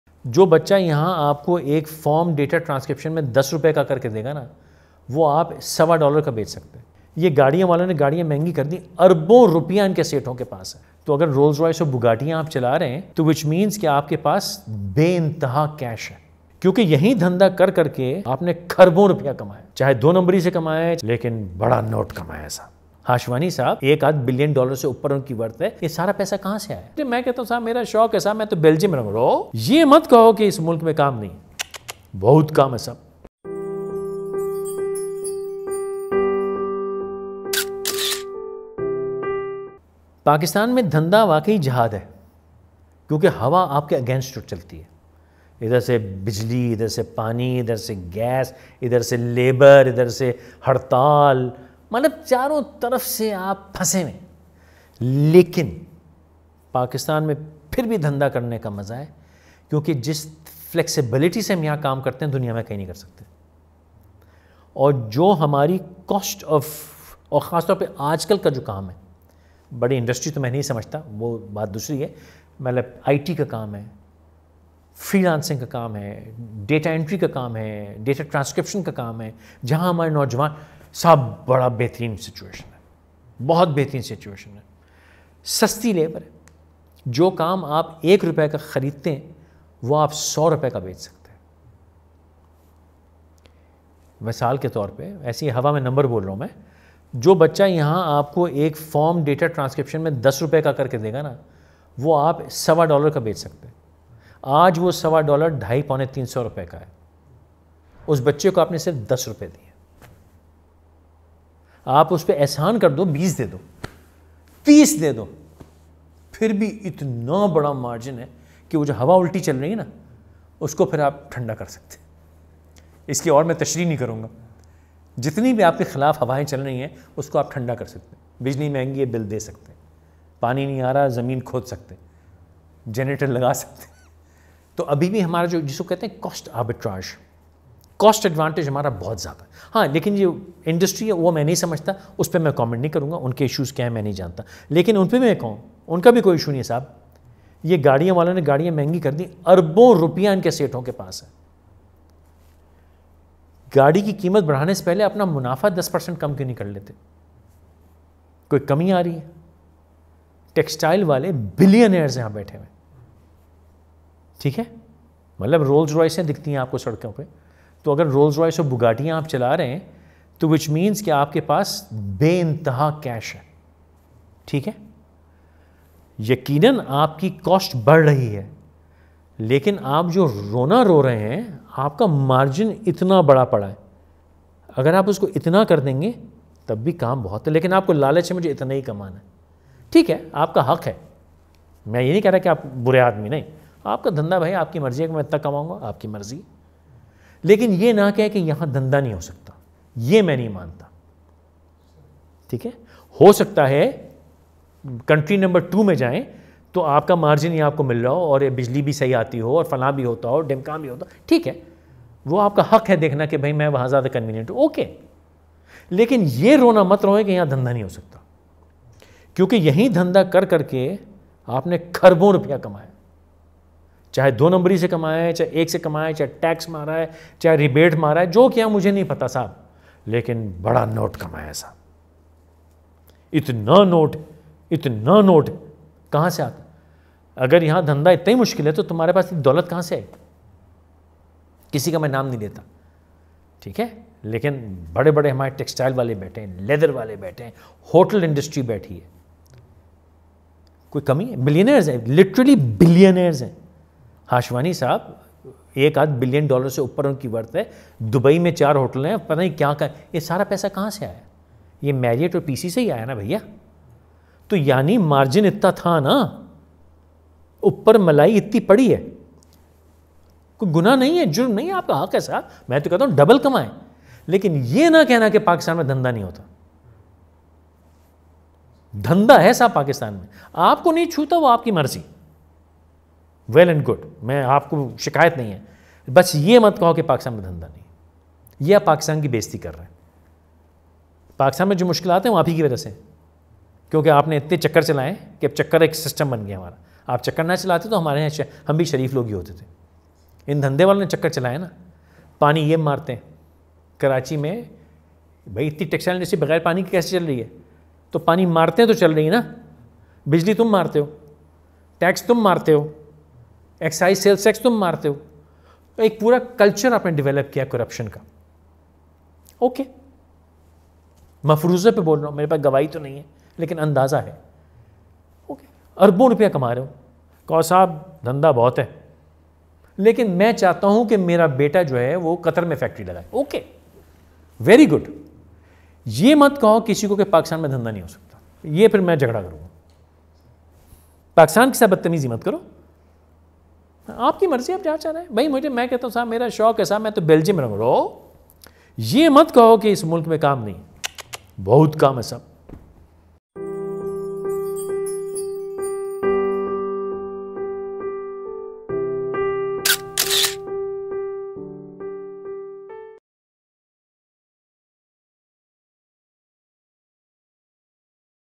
Why Pakistan is still the best place to do business | Flashback Zindagi of Faisal Qureshi
@faisalqureshiis a Pakistani journalist, broadcaster, TV Anchor, Entrepreneur, and Personality Coach.